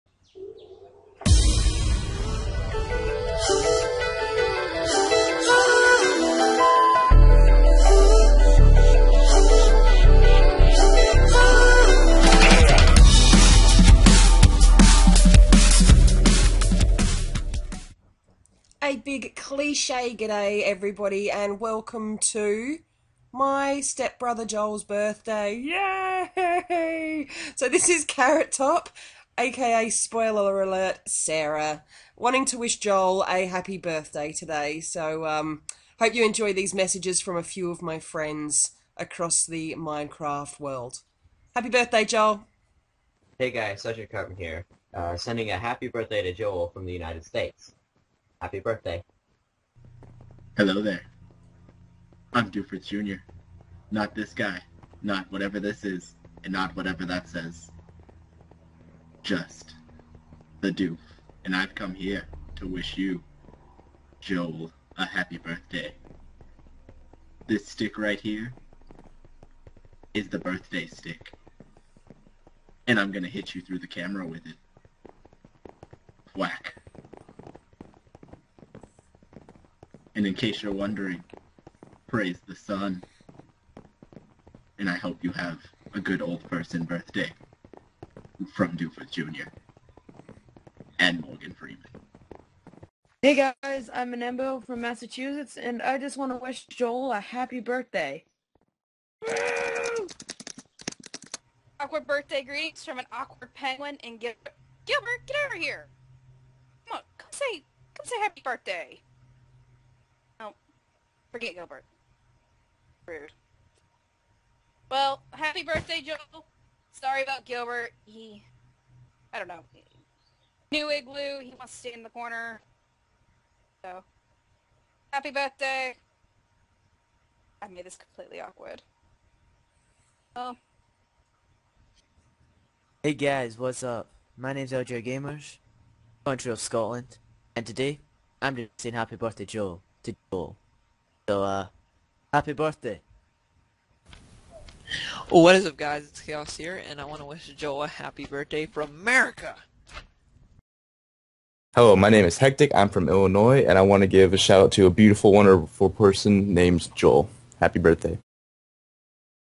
Happy Birthday Joel
Cliché G'day and welcome to a video with a difference.

This vid is going out to my Step Bro, Joel!

I would like to send a big thankyou to all my friends who got involved, please check out their channels, like their vids and subscribe to them, It's soooo worth it!!

From start to end.